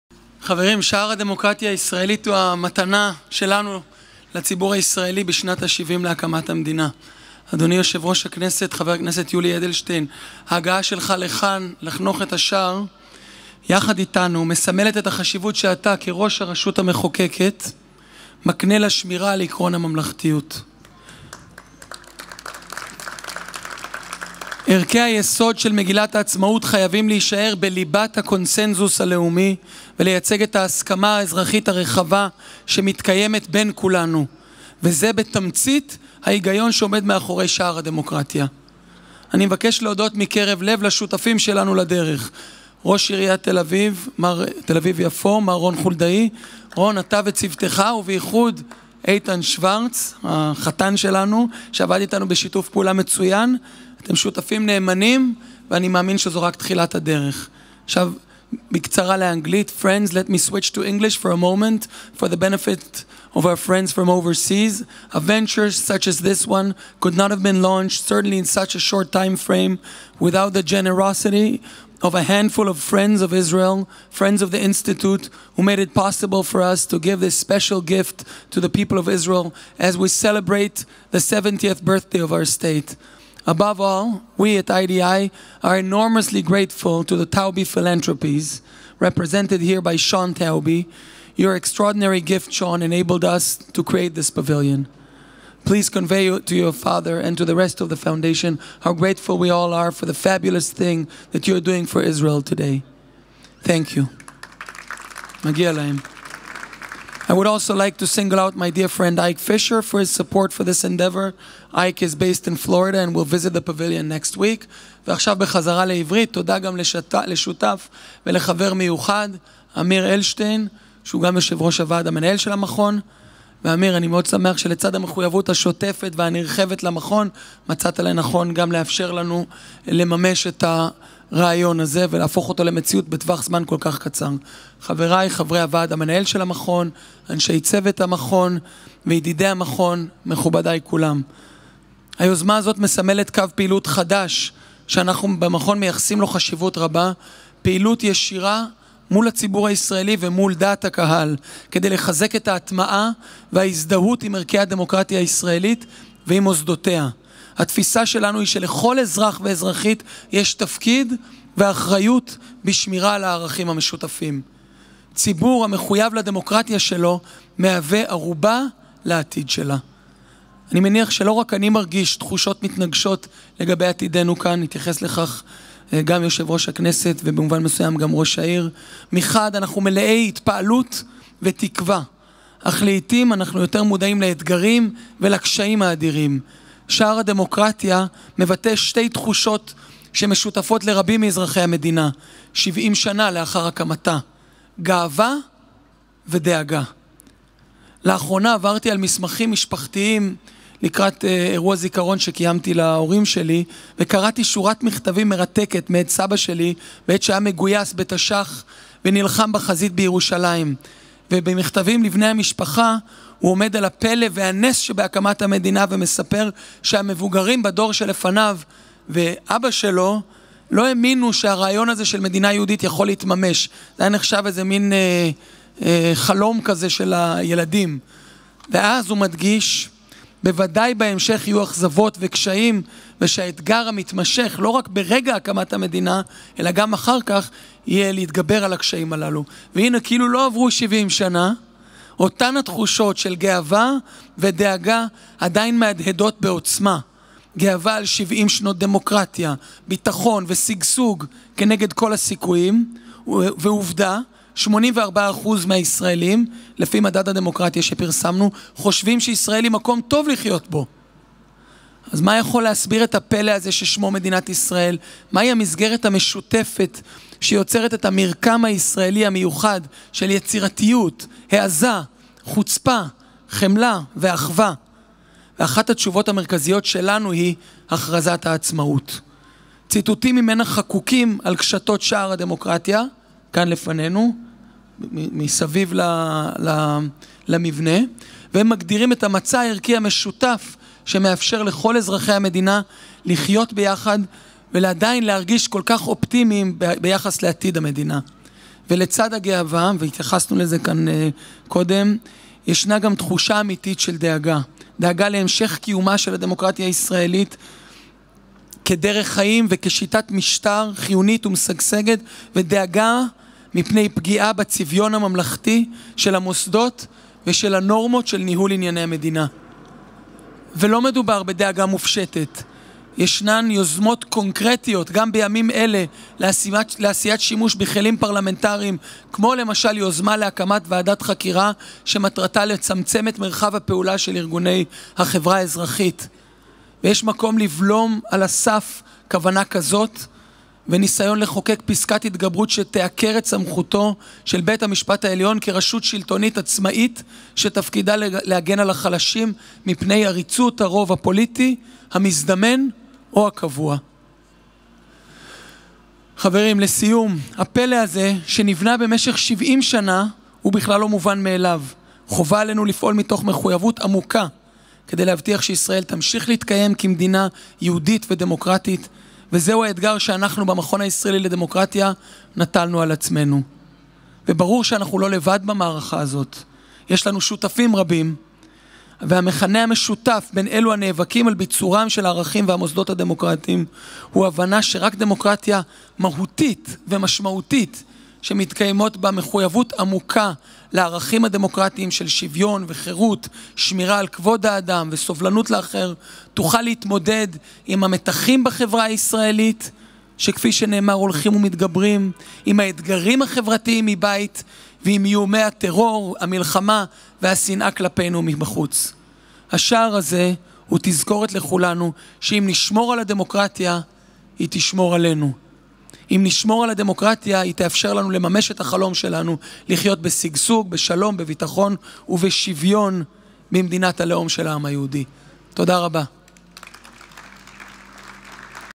נשיא המכון הישראלי לדמוקרטיה, יוחנן פלסנר
יו"ר הכנסת, ח"כ יולי אדלשטיין, ראש עיריית תל אביב-יפו, רון חולדאי ונשיא המכון הישראלי לדמוקרטיה, יוחנן פלסנר חנכו את מיזם "שער הדמוקרטיה הישראלית", מבנה חדשני שבו יוצג סרטון ב-360 מעלות על האירועים שליוו את מדינת ישראל ועל הערכים שבבסיס מגילת העצמאות. המתחם הנמצא ברחוב רוטשילד 1 בת"א, יפתח לקהל הרחב בחינם ערב יום העצמאות

יוחנן פלסנר, נשיא המכון הישראלי לדמוקרטיה: "ערכי היסוד של מגילת העצמאות חייבים להישמר בליבת הקונצנזוס הלאומי וזה, בתמצית, ההיגיון שעומד מאחורי שער הדמוקרטיה. המיזם הזה נועד לחזק את השמירה על הערכים המשותפים לכולנו ולפנות לכל אחד בציבור בכדי שנהיה ציבור המחויב לדמוקרטיה שלו".
פלסנר הוסיף, כי: "הכרזת העצמאות היא אחד האלמנטים שיוצרים את הישראליות, זהו המצע הערכי המשותף שמאפשר לכל אזרחי המדינה לחיות ביחד ולהרגיש אופטימיים ביחס לעתיד המדינה. עם זאת, ישנה תחושת דאגה להמשך קיומה של הדמוקרטיה הישראלי כשיטת משטר ופגיעה בצביון הממלכתי של המוסדות והנורמות של ניהול ענייני המדינה. לא מדובר בסוגיה מופשטת, בימים אלו ישנן יוזמות קונקרטיות כמו היוזמה לוועדת חקירה שתצמצם את מרחב הפעילות של ארגוני חברה אזרחית או הניסיון לחוקק פסקת התגברות שתעקר את תפקודו של הבית המשפט העליון כמגן החלשים. הפלא הזה שנבנה במשך 70 שנה אינו מובן מאליו וחובה עלינו לפעול בכדי שישראל תמשיך להתקיים כמדינה יהודית ודמוקרטית".